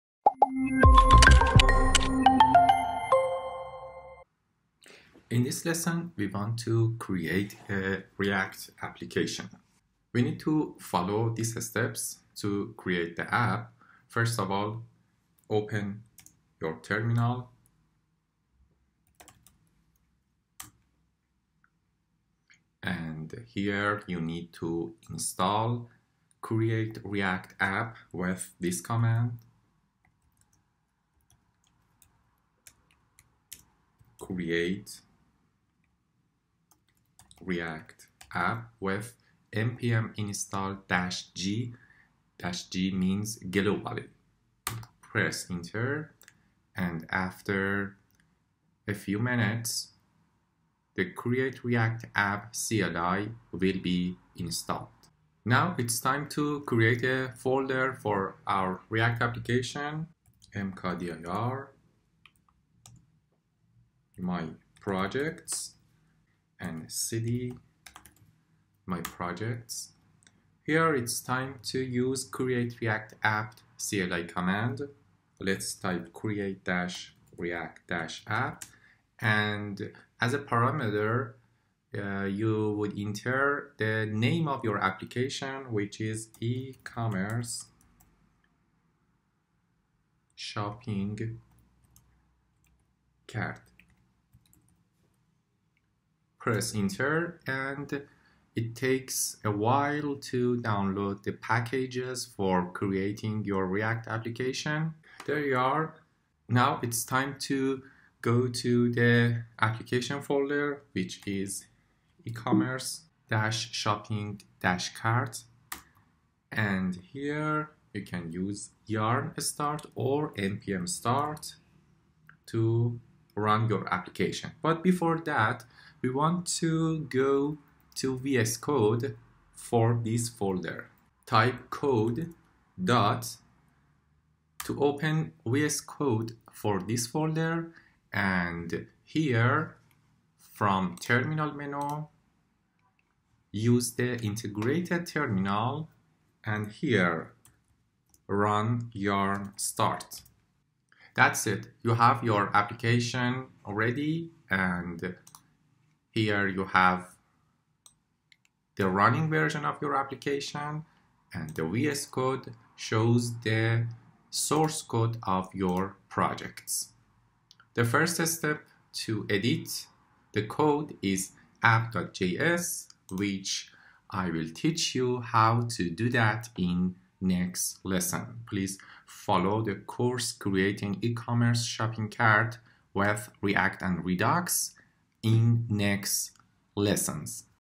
Build React & Redux Shopping Cart Part 02 Creating React App
Learn React & Redux From 0 to 100 by building an E-commerce Shopping Cart Application